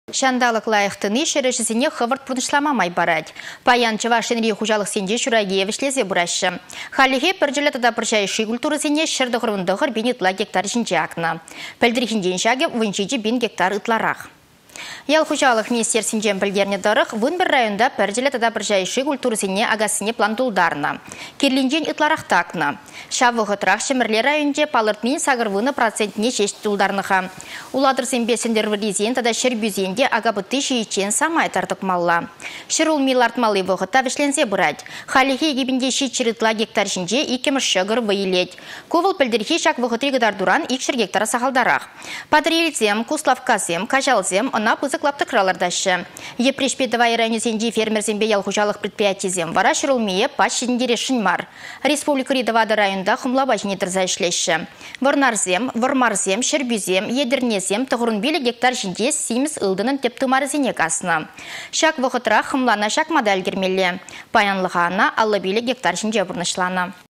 Çураки вĕçленнĕренпе пĕрех
Çанталăк лайăх тăни çĕр ĕçĕсене хăвăрт пурнăçлама май парать. Паян Чăваш Енри хуçалăхсенче çуракие вĕçлесе пыраççĕ. Хальлĕхе пĕрчĕлле тата пăрçа йышши культурăсене 199 пин ытла гектар çинче акнă. Пĕлтĕрхинчен çакă 17 пин гектар ытларах.